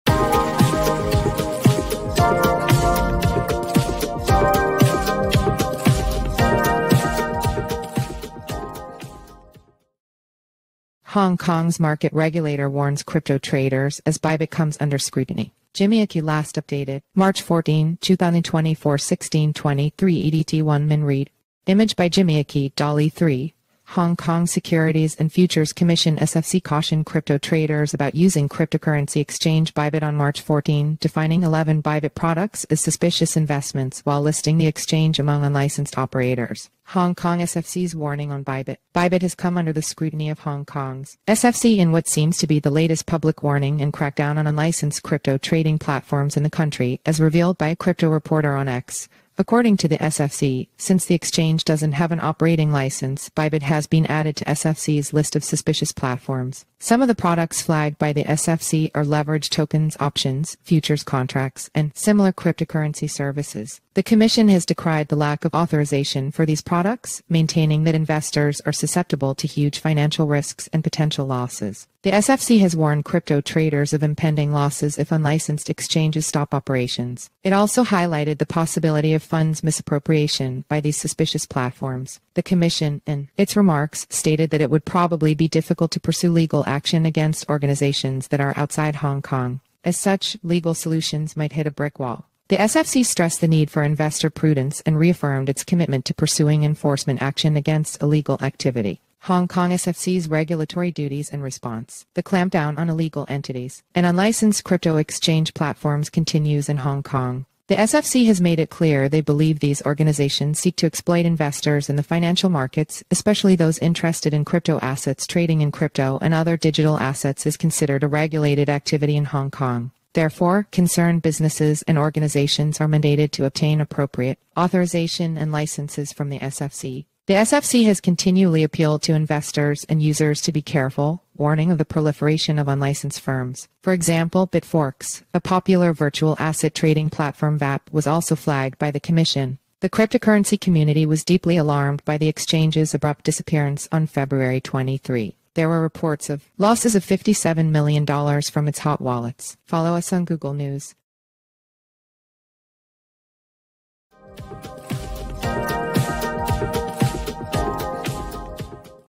Hong Kong’s Market Regulator Warns Crypto Traders, as Bybit Comes
Jimmy Aki

                                                            Last updated:

                              March 14, 2024 16:23 EDT

Image by Jimmy Aki, DALL-E 3.

Hong Kong’s Securities and Futures Commission (SFC) cautioned crypto traders about using cryptocurrency exchange Bybit on March 14, defining 11 Bybit products as suspicious investments while listing the exchange among unlicensed operators.

Hong Kong SFC’s Warning on Bybit.

Bybit has come under the scrutiny of Hong Kong’s SFC in what seems to be the latest public warning and crackdown on unlicensed crypto trading platforms in the country, as revealed by a crypto reporter on X.

According to the SFC, since the exchange doesn’t have an operating license, Bybit has been added to SFC’s list of suspicious platforms.

Some of the products flagged by the SFC are leveraged tokens, options, futures contracts, and similar cryptocurrency services.

The commission has decried the lack of authorization for these products, maintaining that investors are susceptible to huge financial risks and potential losses.

The SFC has warned crypto traders of impending losses if  unlicensed exchanges stop operations. It also highlighted the possibility of funds misappropriation by these suspicious platforms.

The commission, in its remarks, stated that it would probably be difficult to pursue legal action against organizations that are outside Hong Kong. As such, legal solutions might hit a brick wall.

The SFC stressed the need for investor prudence and reaffirmed its commitment to pursuing enforcement action against illegal activity.

Hong Kong SFC’s Regulatory Duties and Response.

The clampdown on illegal entities and unlicensed crypto exchange platforms continues in Hong Kong. The SFC has made it clear they believe these organizations seek to exploit investors in the financial markets, especially those interested in crypto assets

Trading in crypto and other digital assets is considered a regulated activity in Hong Kong. Therefore, concerned businesses and organizations are mandated to obtain appropriate authorization and licenses from the SFC.

The SFC has continually appealed to investors and users to be careful, warning of the proliferation of unlicensed firms. For example, BitForex – a popular Virtual Asset Trading Platform (VATP), was also flagged by the commission.

The cryptocurrency community...